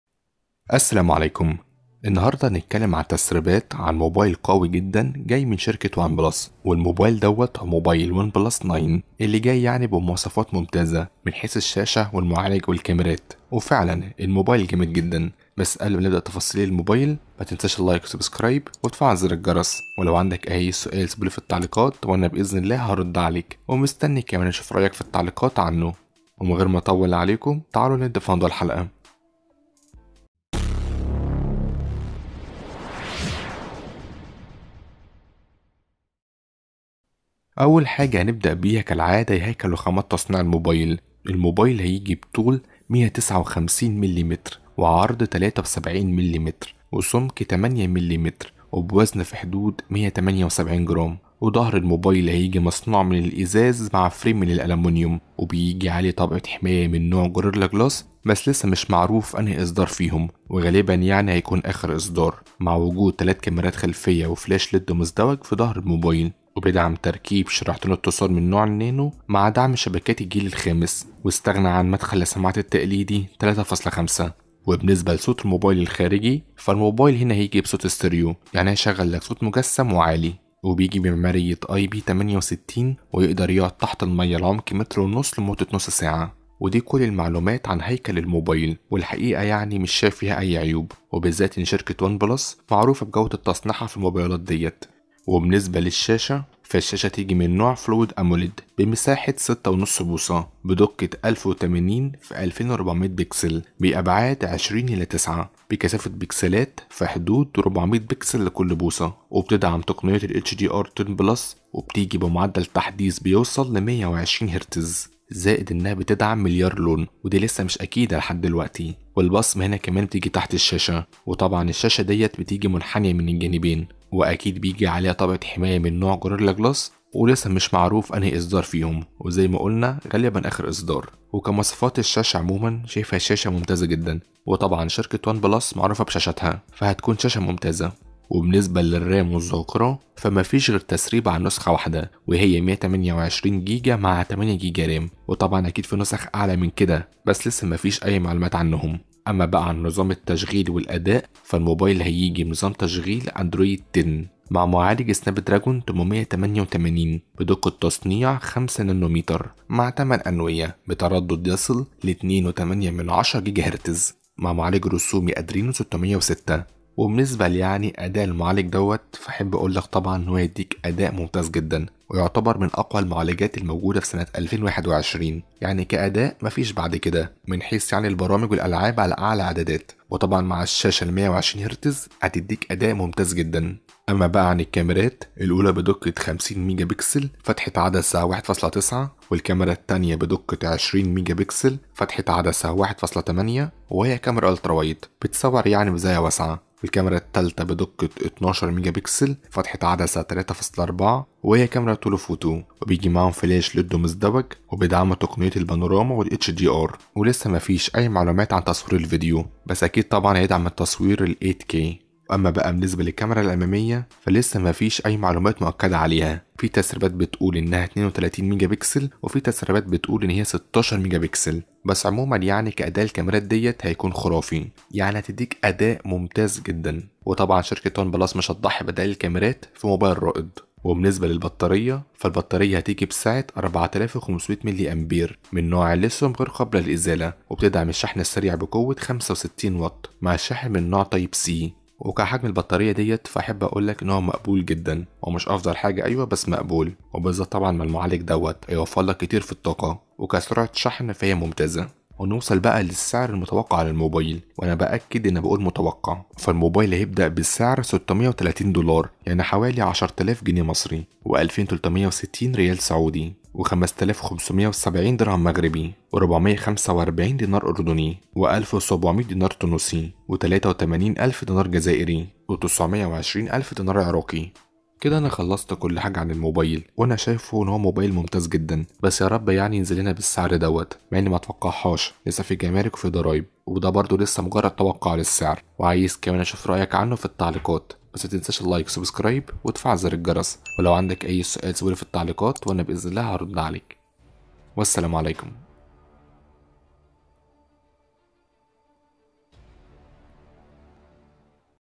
سعر ومواصفات Oneplus 9 - هل سيكون الافضل في عام 2021
السلام عليكم
لم اكتب المواصفات في الوصف لعدم التاكد منها بشكل كافي
السعر التوقع لهاتف Oneplus 9 :
630$ دولار امريكي
10,000 جنيه مصري
2,360 ريال سعودي
5,570 درهم مغربي
445 دينار اردني
1,700 دينار تونسي
83,000 دينار جزائري
920,000 دينار عراقي